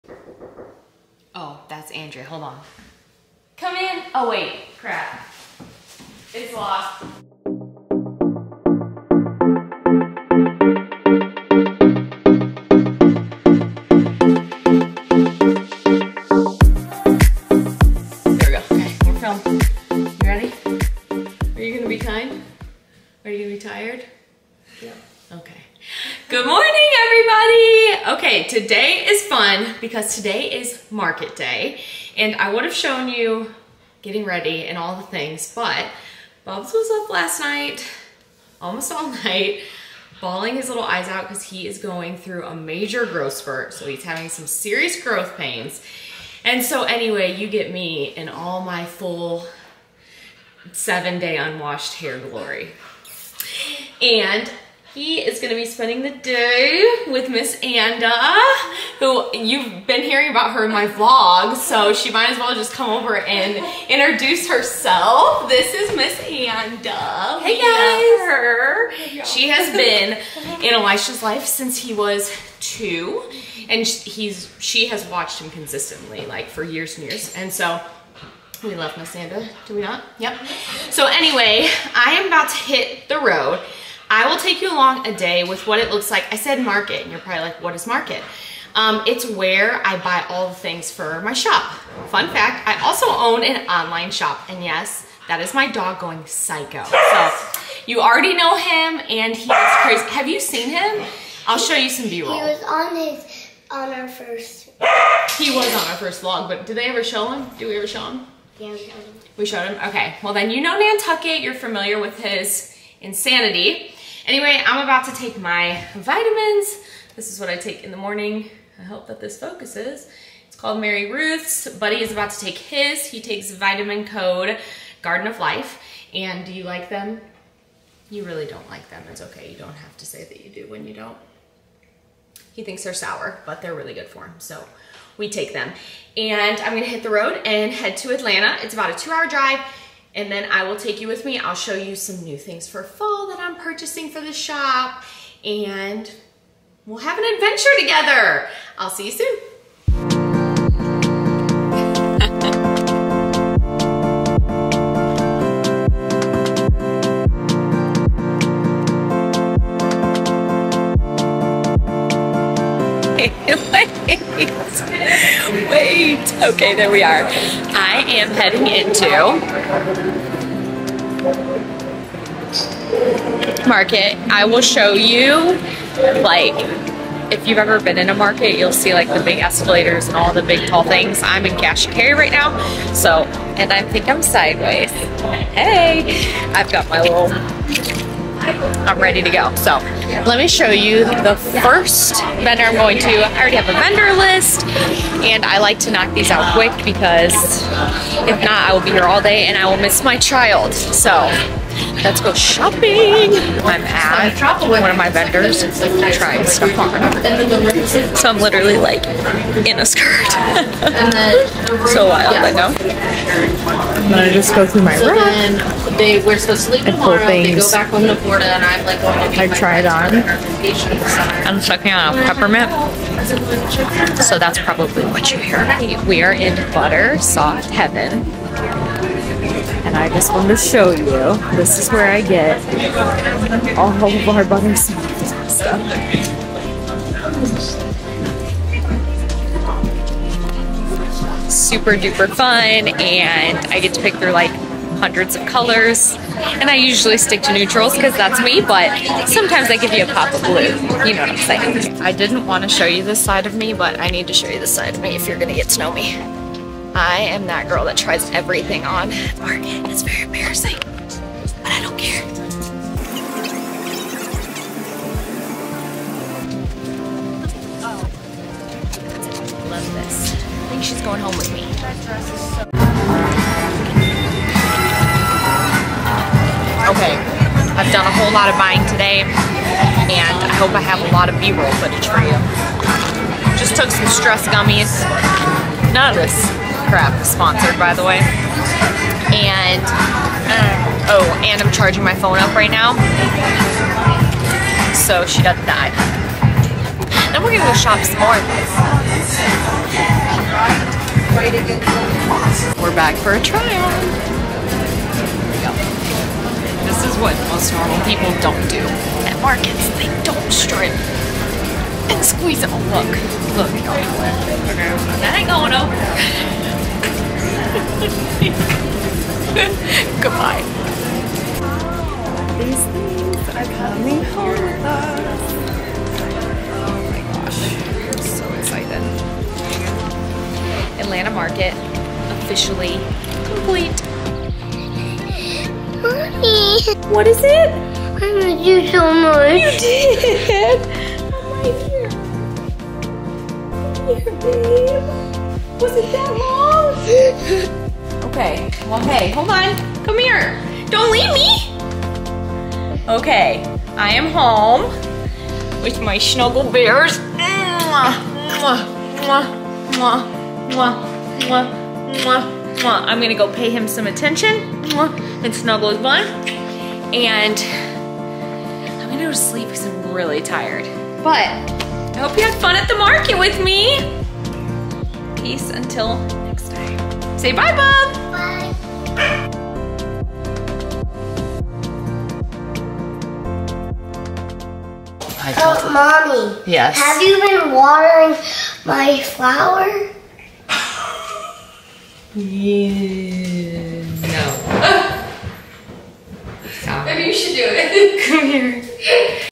Atlanta Market for Shop Elisha
A day at market with an online shop owner : )